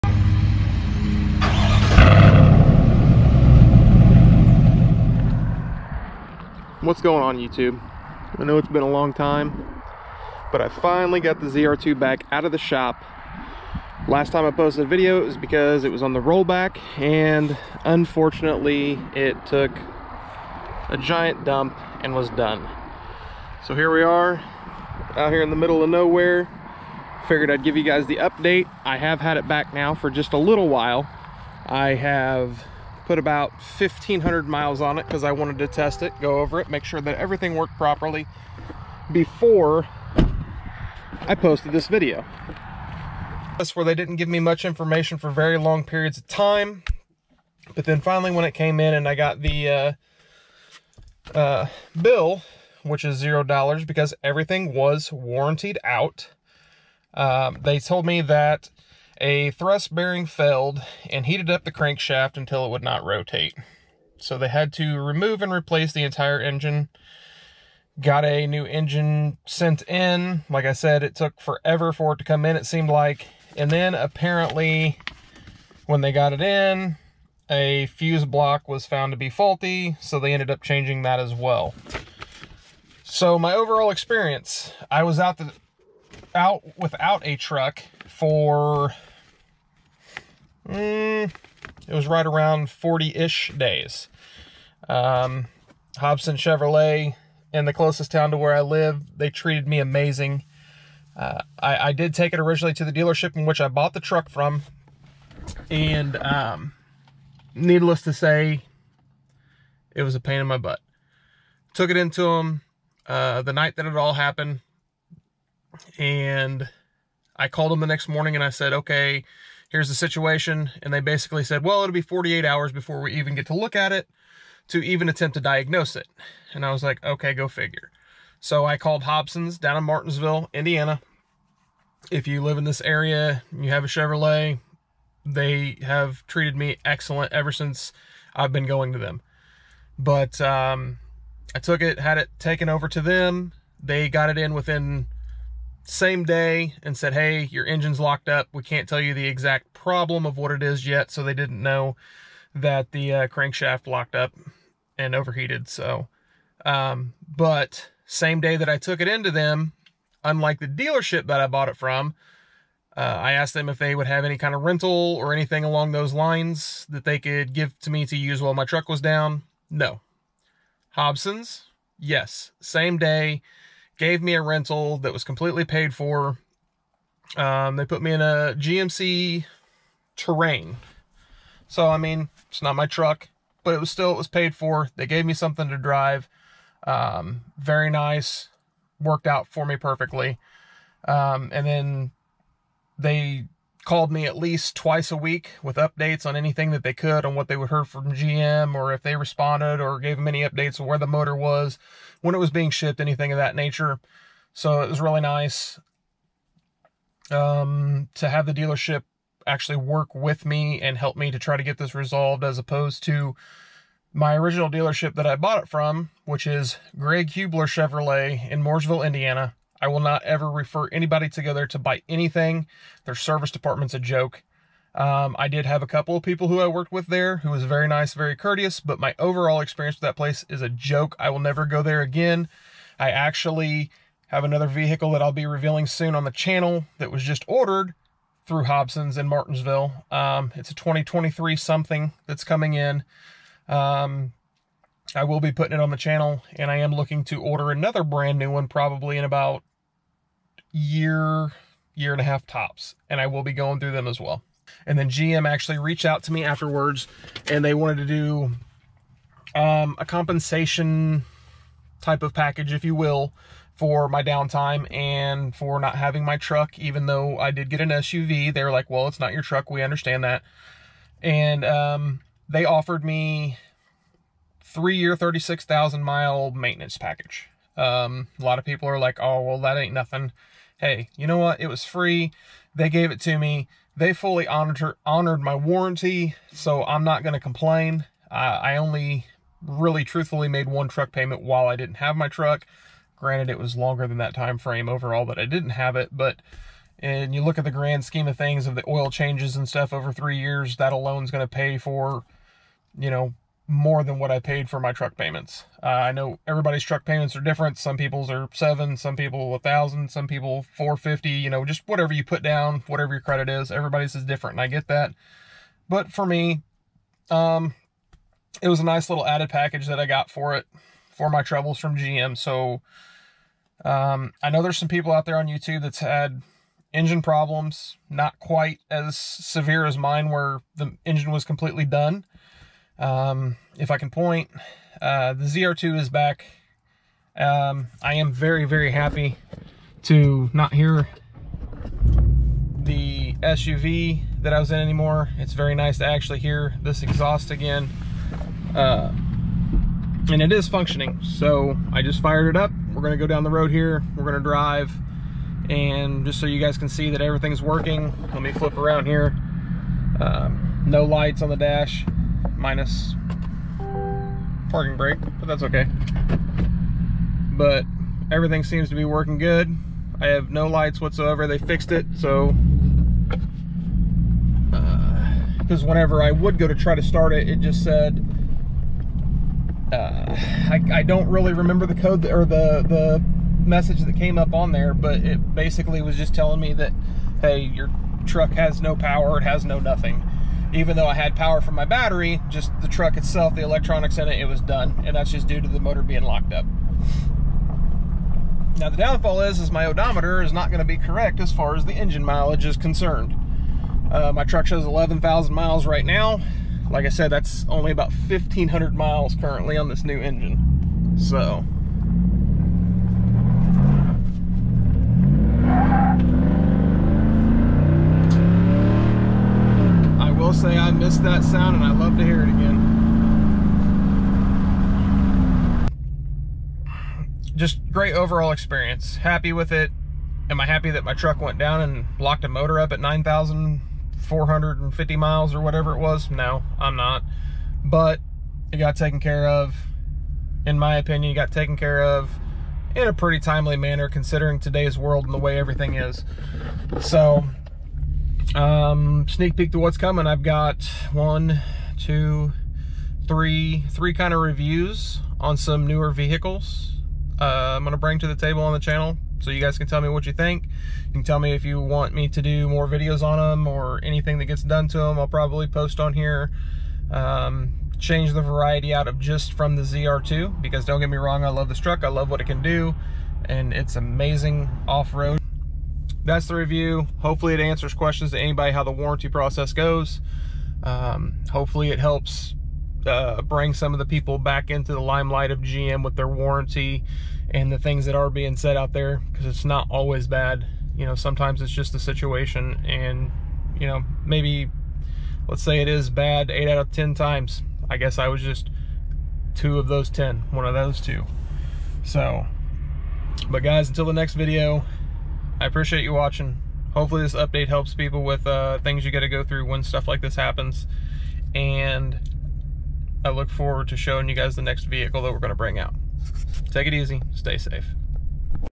GM Warranty really did this.
The conclusion of what happened to the ZR2 after GM Warranty.